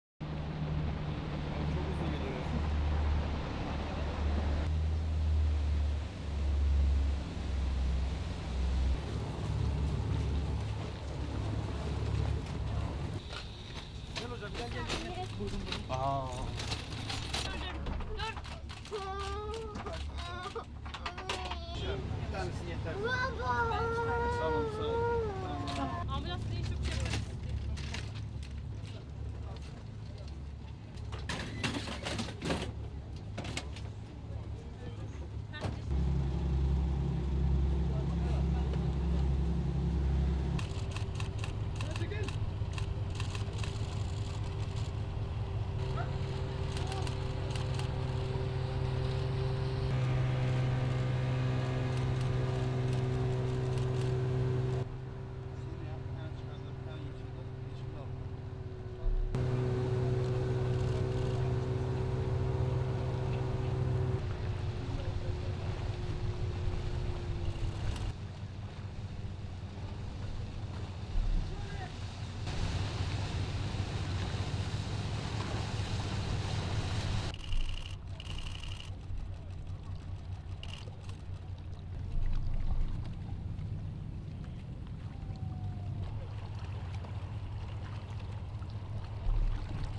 Coast guard helps injured migrants
(18 Aug 2015) Five people have drowned off the Turkish coast as they tried to reach the Greek islands, underscoring the deadly risks of making even short crossings to Europe in overcrowded plastic dinghies.
Turkish coast guards unload five body bags from a patrol boat early on Tuesday as rescued migrants sat on the wharf.
A wailing young boy and a man were put into ambulances as survivors looked on.
Details of the nationalities and identities of the dead were not immediately available.